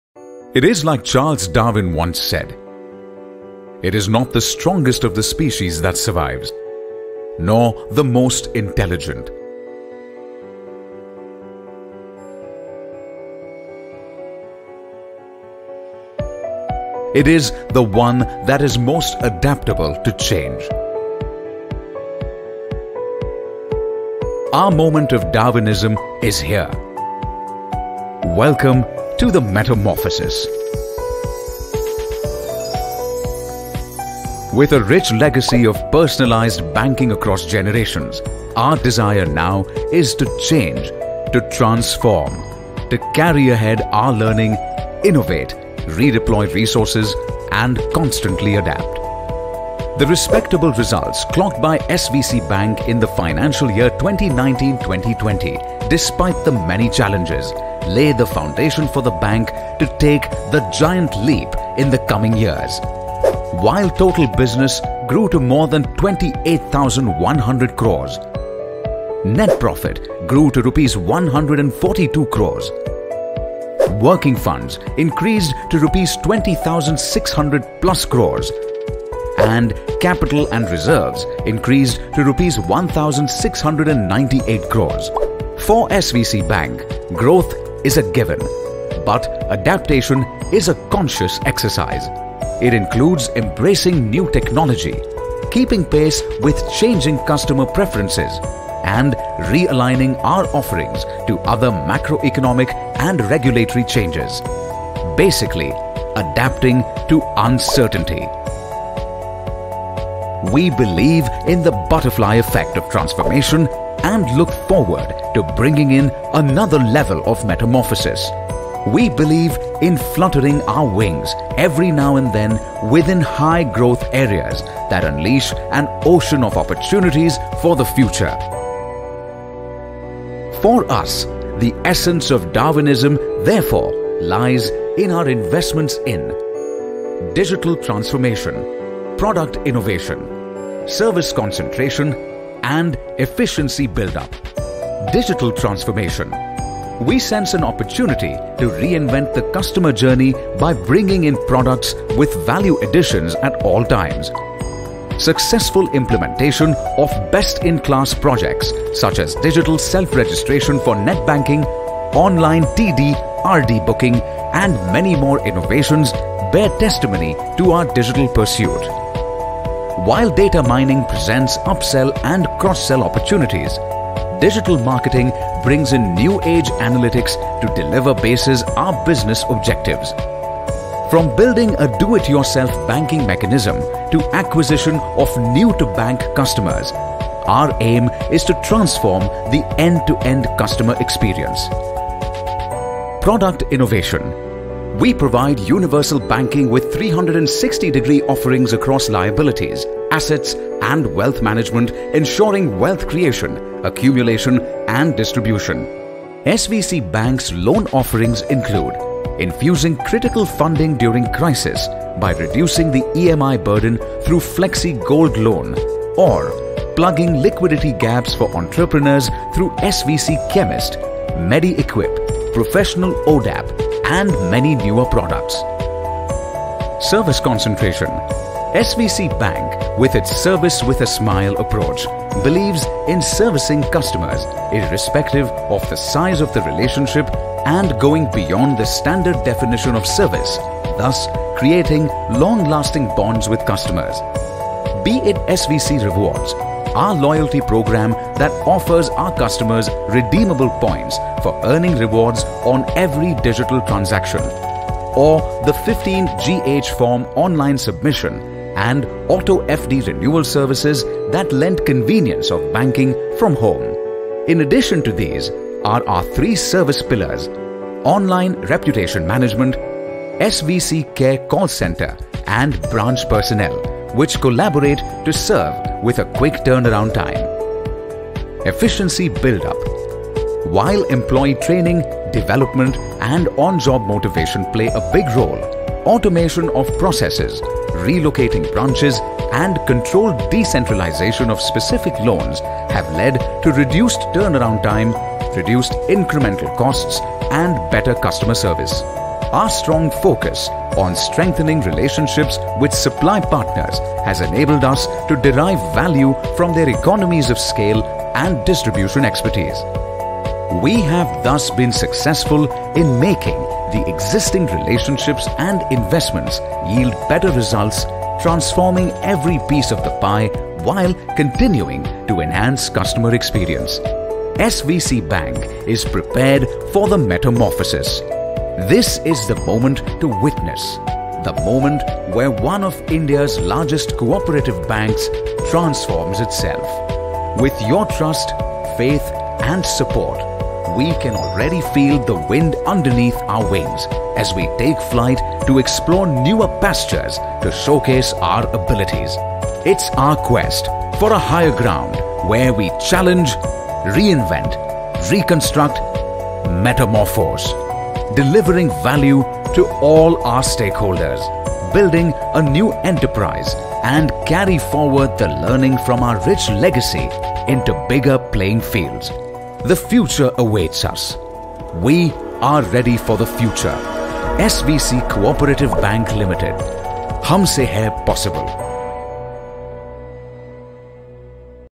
SVC Bank Annual Report 2019 - 2020
This is the Metamorphosis - The quest for a higher ground.
Watch the Butterfly Effect on Transformation take shape for SVC Bank, as it takes flight into the future.